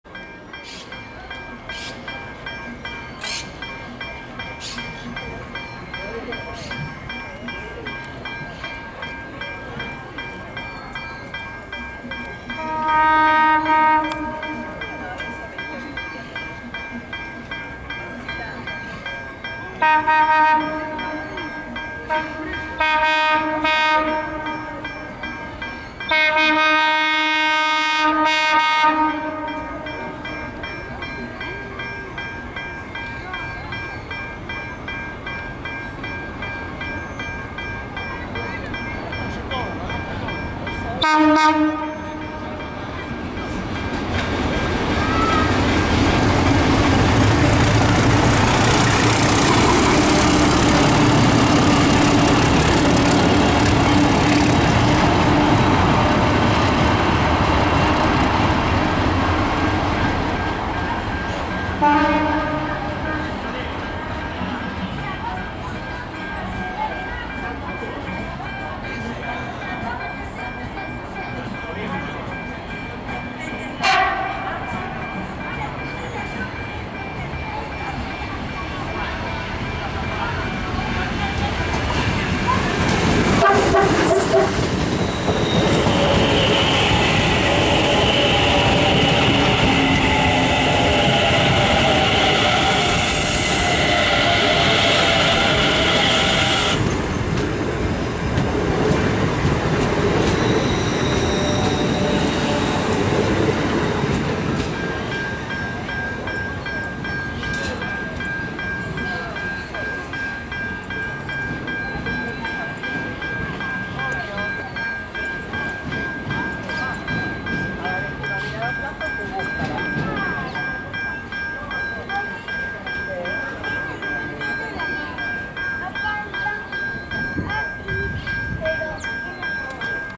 ALCo DL535 659 y 664 por Belgrano R
Locomotoras ALCo DL535 D659 y 664 de Trenes Argentinos haciendo su paso por Belgrano R, provenientes de Retiro y con destino a deposito Suarez. También podremos observar formación eléctrica CSR SFM-23 M17 proveniente de Suarez y con destino a Retiro.